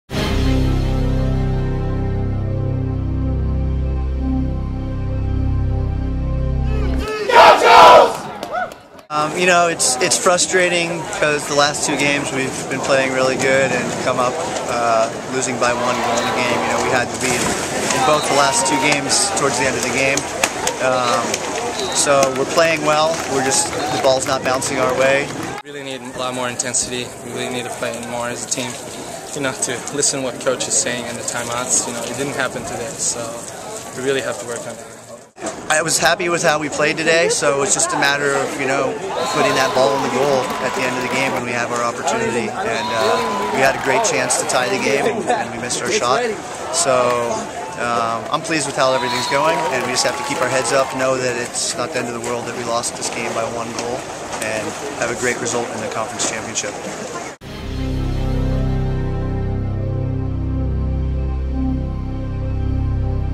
Men's Water Polo: 11/14: Recap_UCLA
#7 UCSB Men's Water Polo falls short to #4 UCLA on Sunday afternoon. UCLA 9, UCSB 8.